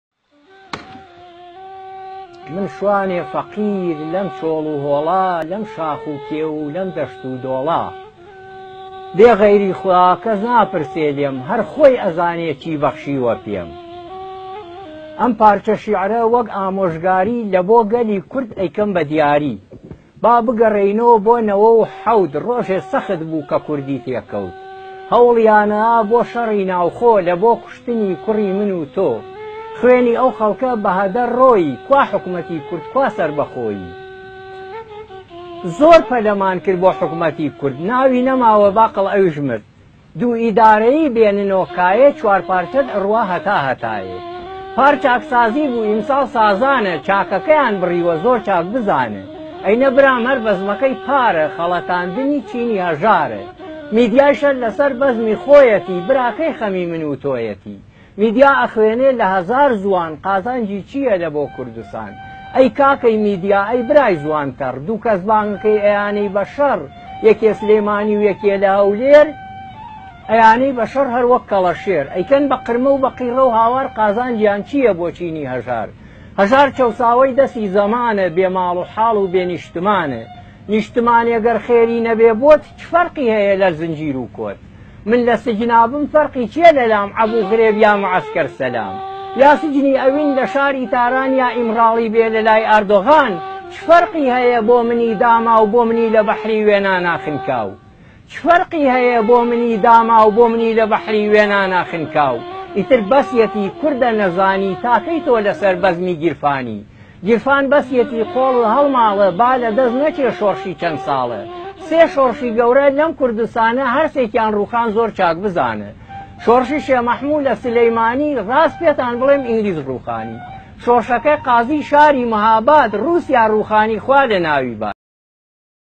شعر تنها اتحاد چاره درد کورد است
شعری از شاعر کرد باشور کوردستان